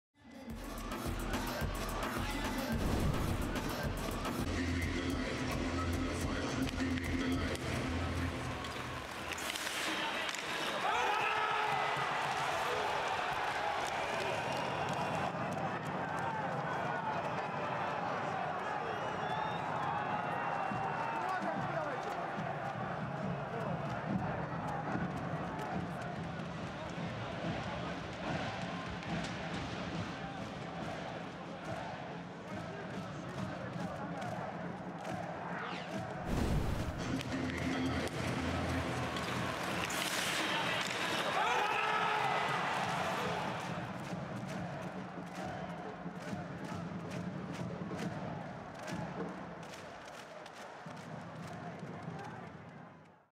Ковальчук подправляет бросок Рыкова
Илья Ковальчук открывает счёт в матче